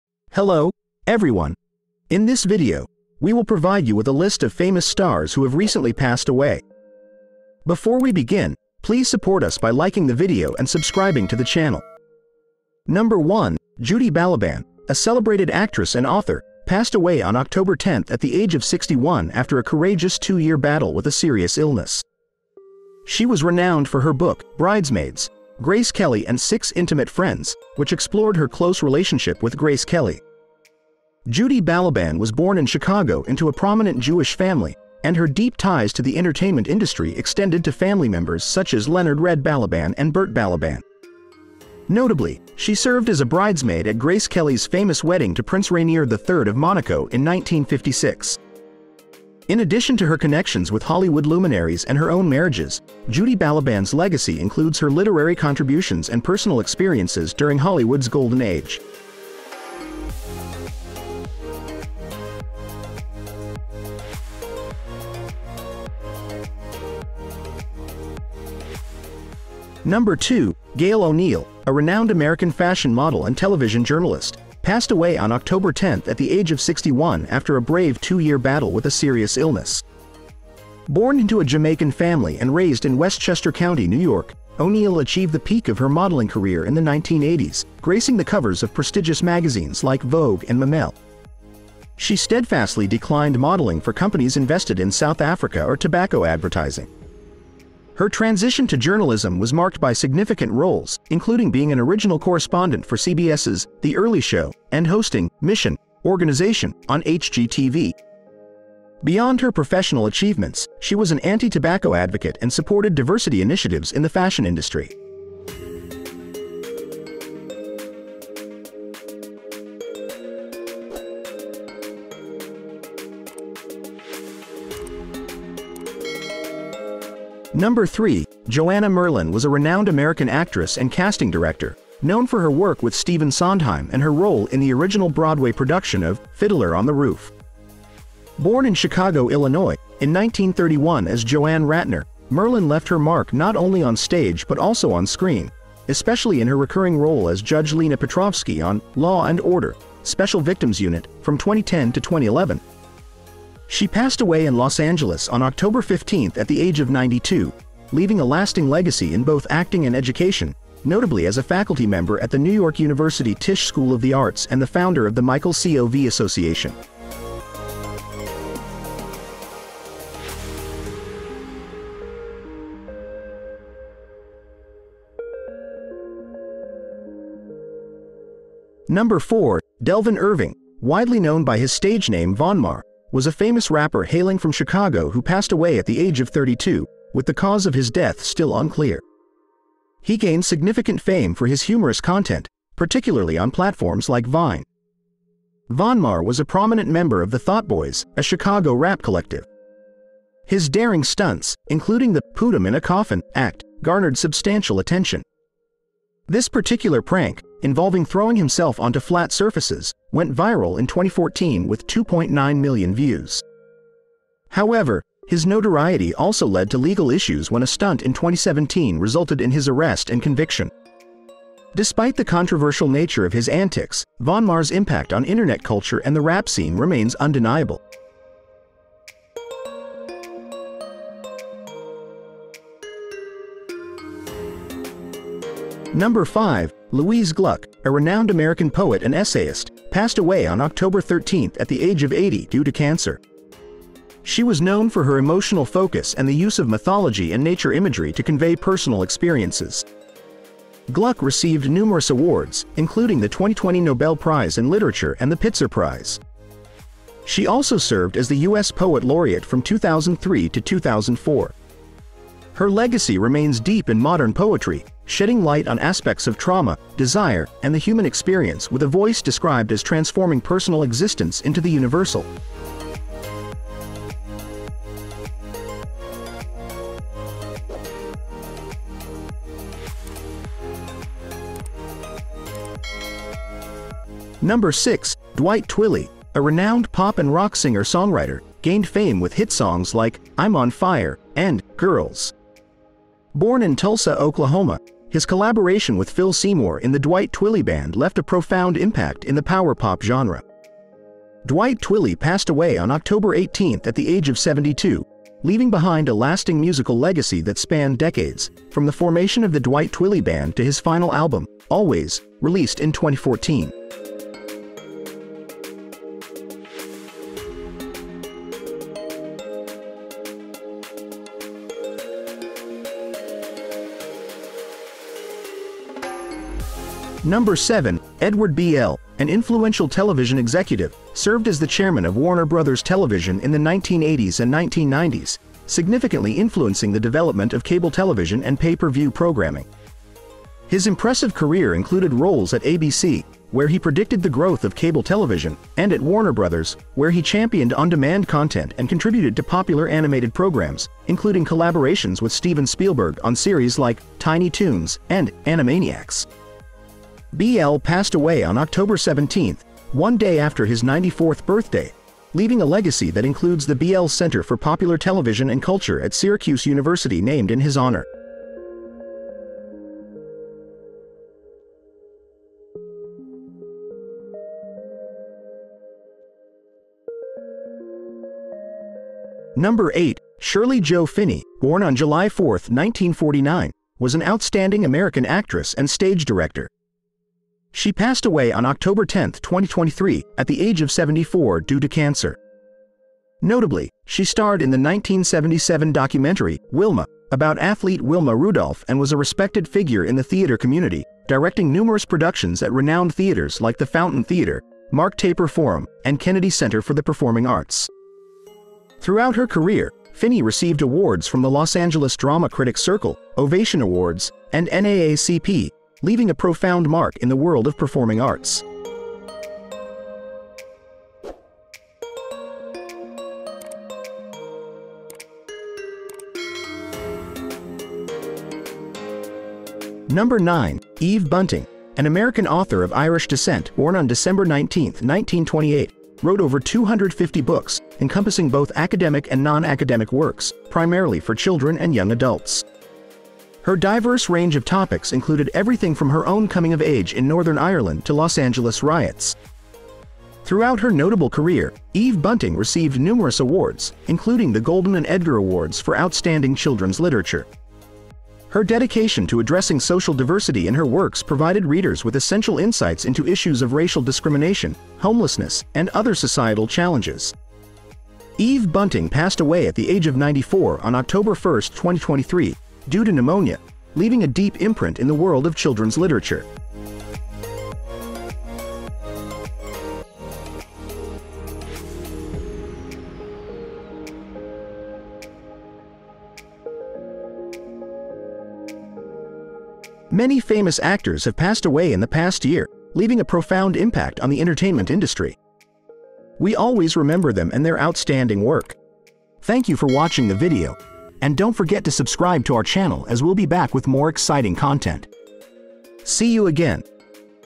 Big STARS WHO DIED in the Last 24 Hours, Cast Then and Now 2023
Elaine Devry, a beloved actress from Hollywood's golden era, has sadly passed away today in America. Join us as we celebrate her extensive career and the lasting impact she left on the entertainment industry.
Today, we remember Keith Jefferson, the talented actor known for his roles in Quentin Tarantino's films. Explore his contributions to American cinema and the legacy he leaves behind.
Burt Young, the iconic actor, painter, and author, has left us today in America. Let's honor his versatile talents and the indelible mark he made on the arts.
Lara Parker, best known for her role in "Dark Shadows" and her literary achievements, passed away today in America. Discover her multifaceted career and the impact she had on both screen and page.
Dave Courtney, the enigmatic English figure known for his stories of the underworld, left us today in America. Explore his controversial yet influential legacy in British pop culture.
Mark Howard James, also known as The 45 King, passed away today in America. Join us as we celebrate his legendary contributions to hip-hop and his lasting influence on the genre.
Remembering Piper Laurie, the versatile American actress who graced both film and television, as she passed away today in America. Explore her remarkable career and the awards she garnered.
Today, we pay tribute to Haydn Gwynne, the celebrated English actress known for her stage and screen roles, who passed away in America. Discover her contributions to the world of entertainment.
Mark Goddard, the American actor with a multifaceted career in entertainment and education, has left us today in America. Learn about his remarkable journey in the industry.
Judy Balaban, the actress and author with ties to Hollywood royalty, has passed away today in America. Explore her literary contributions and her fascinating connections within the industry.
Gail O'Neill, the prominent fashion model and television journalist who advocated for diversity, passed away today in America. Discover her impact on the fashion world and beyond.
Join us in remembering Joanna Merlin, the actress and casting director, who made her mark on Broadway and television. She passed away today in America, leaving behind a rich legacy.
Delvon Irving, also known as Vonmar, the Chicago rapper and internet sensation, has left us today in America. Explore his unique contributions to rap and internet culture.
Today, we mourn the loss of Louise Glück, the renowned American poet and essayist, who passed away in America. Discover the profound impact she had on modern poetry.
Dwight Twilley, the influential American pop and rock singer-songwriter, has left us today in America. Explore his memorable music career and his iconic hits.
Remembering Edward Bleier, the influential American television executive who shaped the cable TV landscape. Join us as we celebrate his contributions to the entertainment industry.
Shirley Jo Finney, the accomplished American actress and theater director, has passed away today in America. Explore her impressive career on both stage and screen.
Today, we remember Eve Bunting, the prolific author who made a significant impact on children's literature. Discover her diverse range of books addressing important social issues.